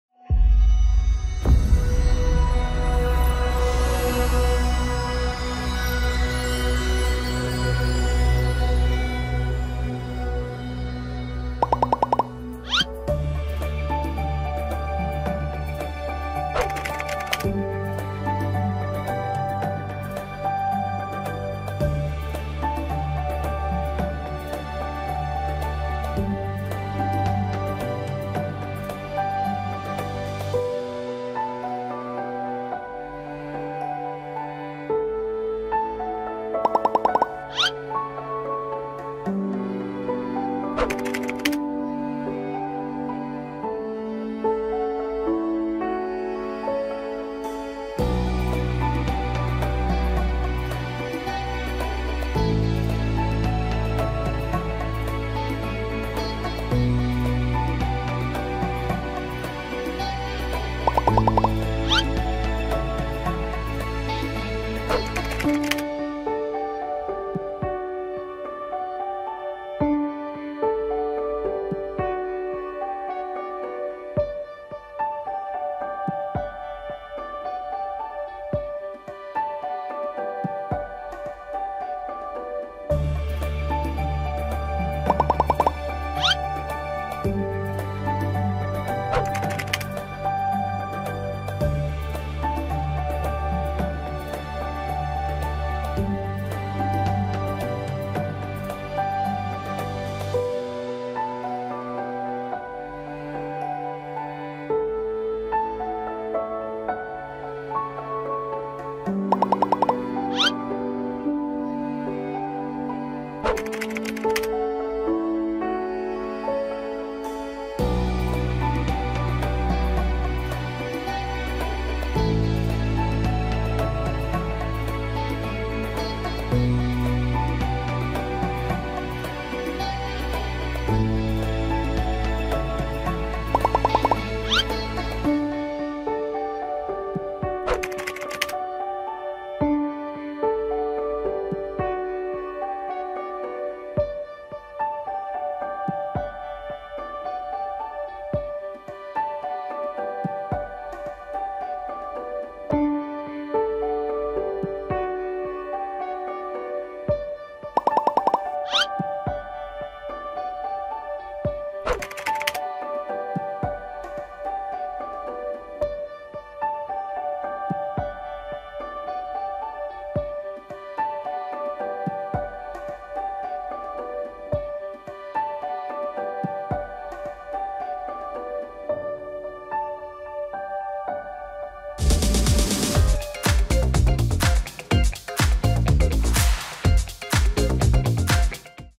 ดวงประจำวันที่ 23 พฤษภาคม 2566 | ข่าวช่อง8
ส่องดวงรายวัน ทั้ง 7 วัน การงาน การเงิน ความรัก รุ่งหรือร่วง รู้ก่อนใครที่นี่ เปิดชมเลย...

ขอบคุณข้อมูล : สมาคมโหรแห่งประเทศไทย ในพระบรมราชินูปถัมภ์